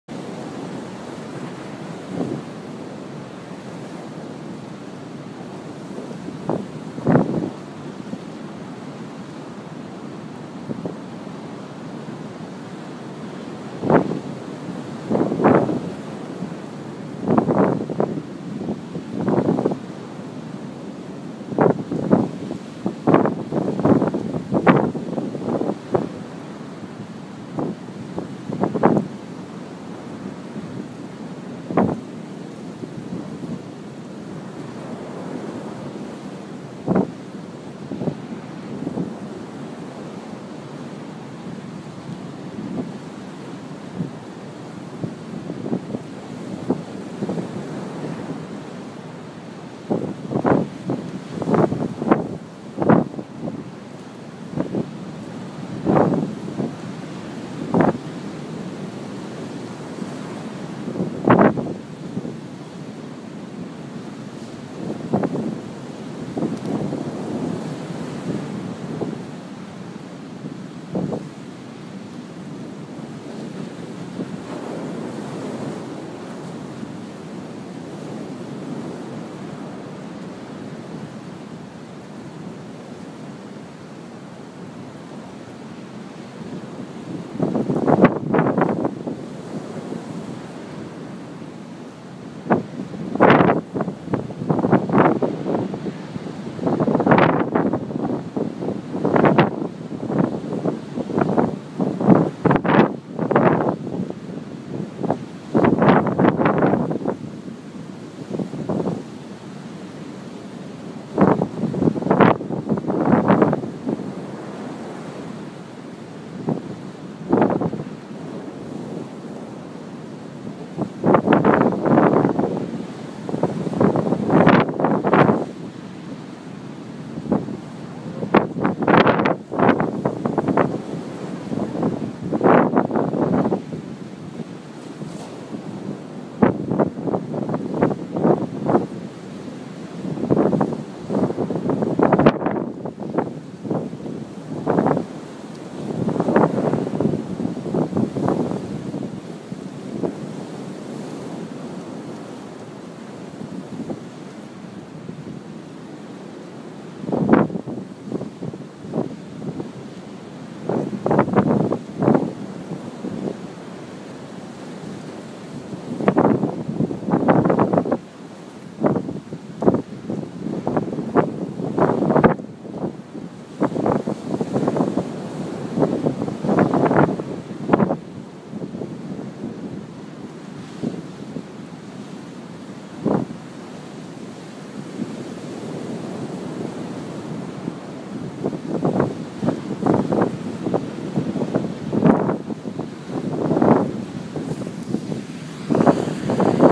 Mr Durban Crab (Short)
This is a video we took of a crab digging his home on the Durban coast of South Africa.
I wanted to share him with the rest of the world so we could all get a glimpse into the lives of the tiny creatures that live around us.

Taken with an iPhone 4s on the 23rd of October 2012